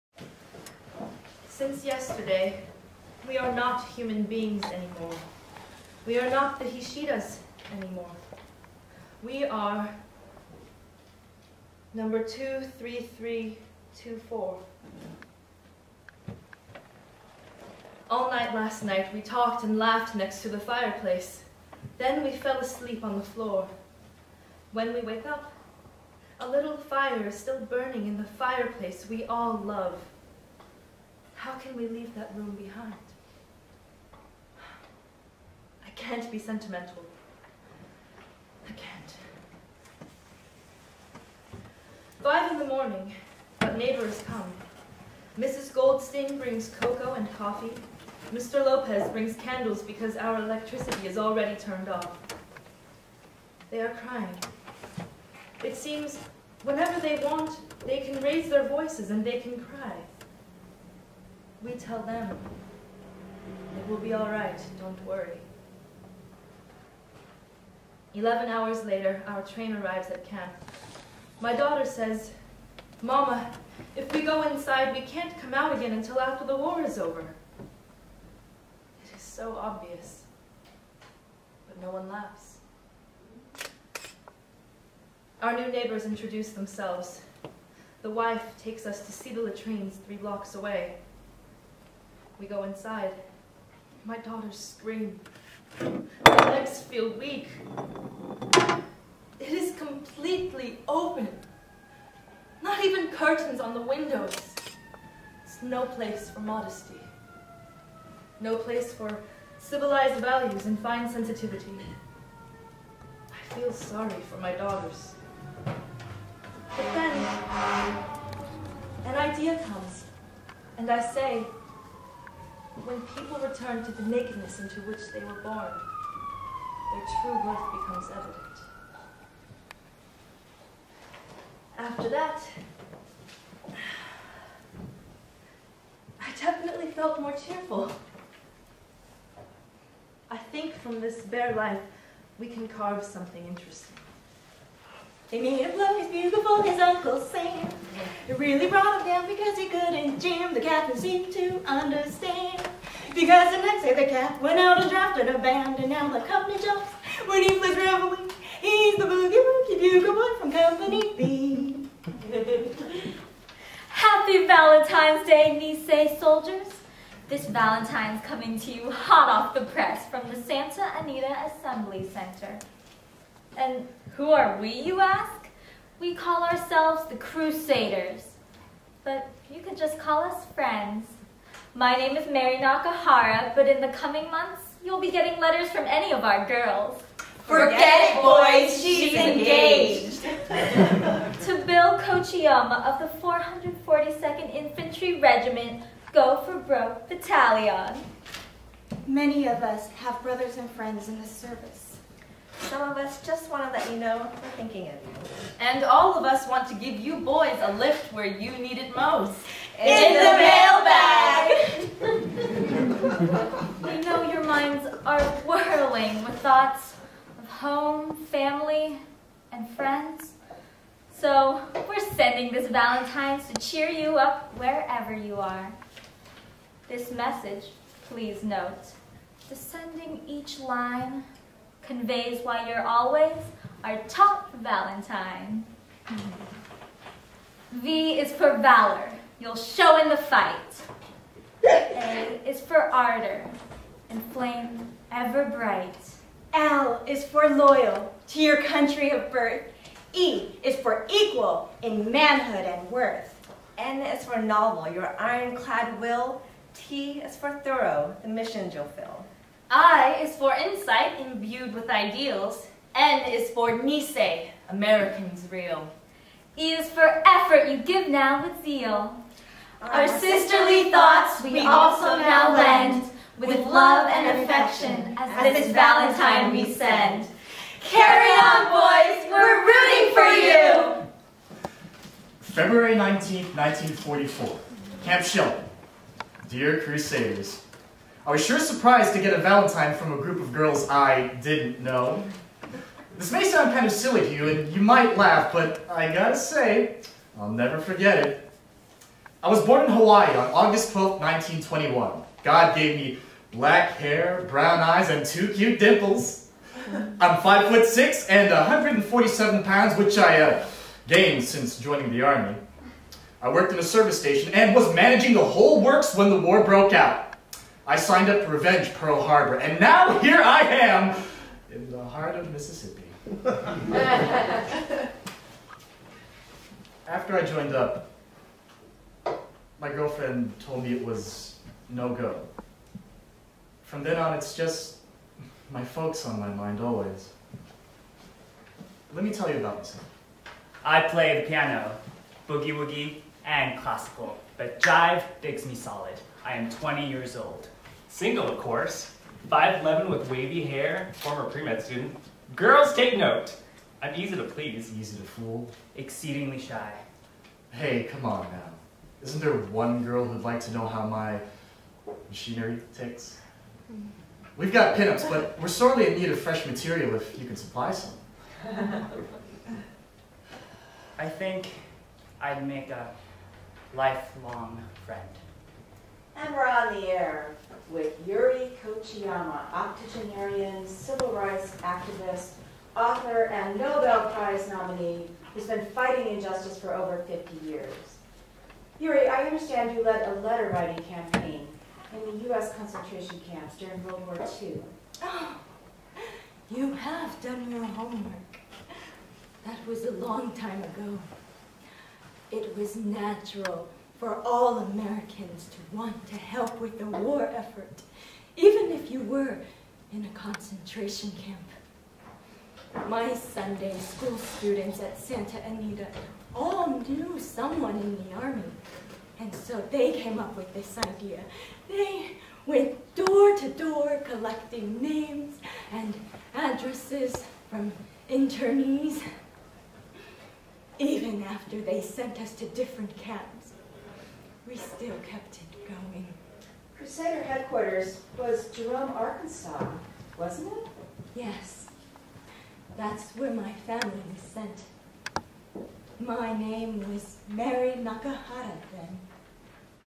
Part 1 Bits of Paradise Nov 17th Performance for Yuri Kochiyama
Before Yuri Kochiyama was nominated for the 05 Nobel Peace Prize and before she cradled Malcolm X's bleeding head, the octogenerian human rights activist was a determined 20-year old who led a World War II letter-writing campaign to boost the morale of Japanese American soldiers. The catch? The morale-boosters were themselves interned in U.S. concentration camps. Together they proved the Canon Westcott saying that Kochiyama quoted at the time in her Pacific Citizen column that "Occasions do not make heroes--they simply unveil them." Playwright/Director Marlan Warren has compiled these letters into a spoken word piece entitled Bits of Paradise. Excerpts starring Chanelle Yang, Pisha Warden, Linda Wang, Connie Kim, Fel Phillip, Wesley Cayabyab, Jean Franco, and Wilton Yiu will be presented at The Marsh. Shot by Ben Kim

Yuri Kochiyama is in the audience